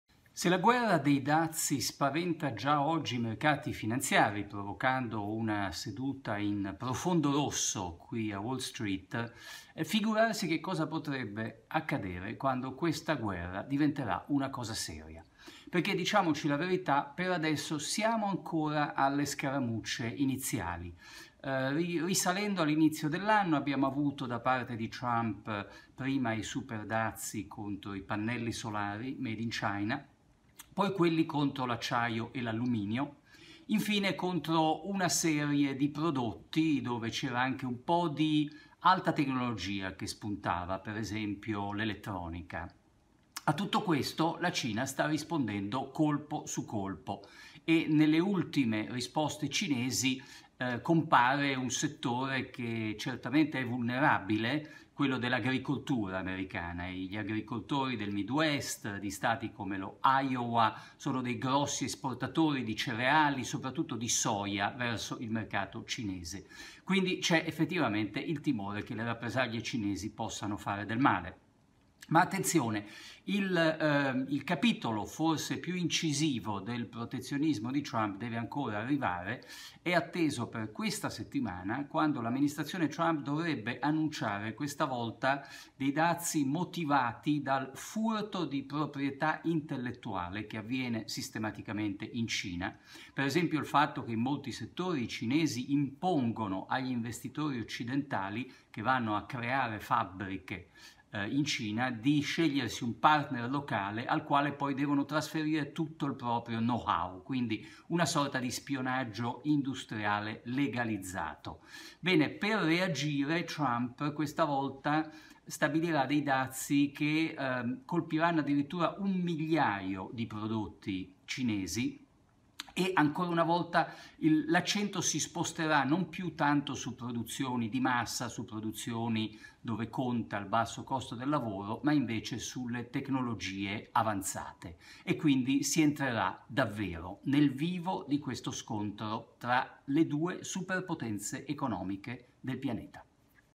Trump e i dazi alla Cina, Rampini: "La vera guerra commerciale deve ancora iniziare"
I rincari, partiti su oltre 120 prodotti importati dagli Usa, fanno parte di una guerra commerciale iniziata con le tariffe maggiorate su acciaio e alluminio made in China. Pesante la reazione a Wall Street. "Siamo alle scaramucce iniziali. Se la guerra dei dazi spaventa i mercati finanziari, figurarsi cosa potrebbe accadere quando questa guerra diventerà una cosa seria, ovvero quando si toccherà il miliardo di prodotti legati al reparto tecnologico". Il commento di Federico Rampini